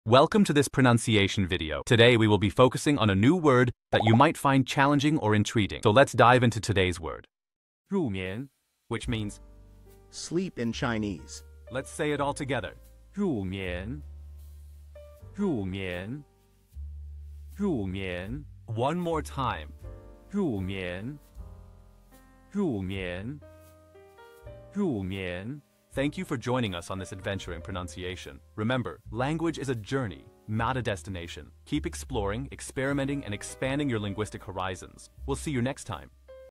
How to pronounce rù mián | 入眠 (Sleep in Chinese)
Master the Pronunciation of 'rù mián | 入眠' - which means : Sleep in Chinese 📝 with @PerfectPronunciationChinese 🎓🗣️ - [Your Guide to Chinese]

Learn to pronounce 'rù mián' perfectly in Chinese with our easy and engaging tutorial by @PerfectPronunciationChinese! 📖📽️ In this comprehensive video, we provide you with native speaker examples 🗣️👂 and phonetic breakdowns 🎵 to ensure you get the pronunciation just right.